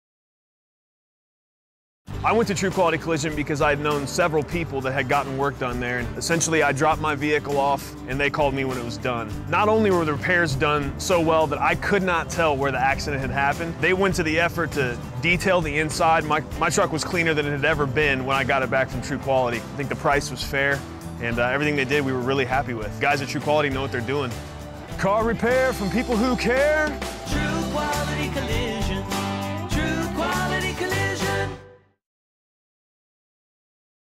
TV Commercial for True Quality Collision - Vincent Testimonial
Testimonial television commercial for True Quality Collision, created by Creative Dimensions, a Greater Cincinnati / Northern Kentucky advertising agency / ad agency.